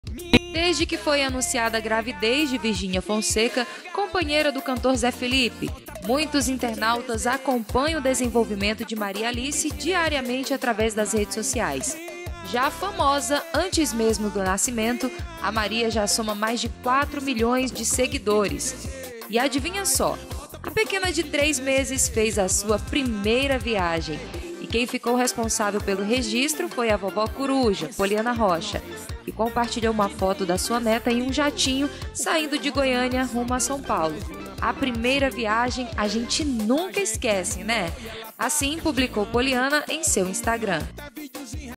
Mãe de Zé Felipe registra primeira viagem de avião de Maria Alice
Poliana Rocha, mulher do cantor Leonardo, compartilhou com seus seguidores a primeira viagem de avião da neta, Maria Alice. A menina, que nasceu no dia 30 de maio, acompanhou os pais, Virginia Fonseca e Zé Felipe, em uma viagem para São Paulo de jatinho.

"A primeira viagem a gente nunca esquece, né!", escreveu a influenciadora na legenda da foto em que aparece segurando a bebê. A avô materna de Maria Alice, Margareth Serrão, também fez um clique do momento.

Famosos, Celebridades, Fofoca, Notícias, Mundo dos famosos, Fofocalizando, Artistas, Musicos,